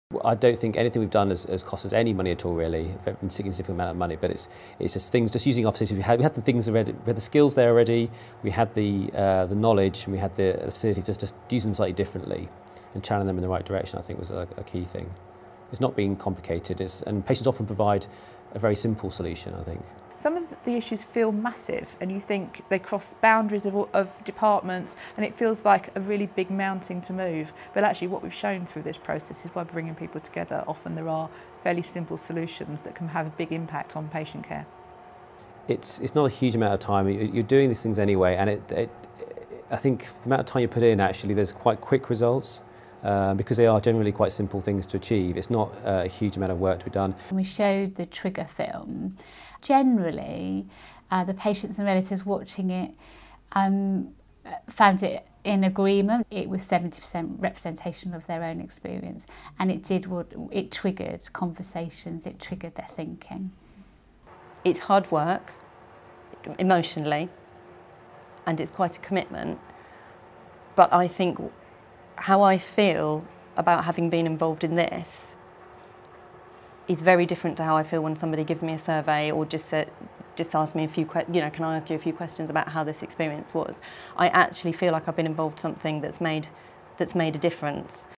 EBCD 15: Adapting the approach to your budget
If you are keen to carry out experience-based co-design (EBCD) but are concerned about the resources required, this video explains which components are essential and which could be adapted.

This video explains how to evaulate your project and celebrate your successes. It is part of a toolkit available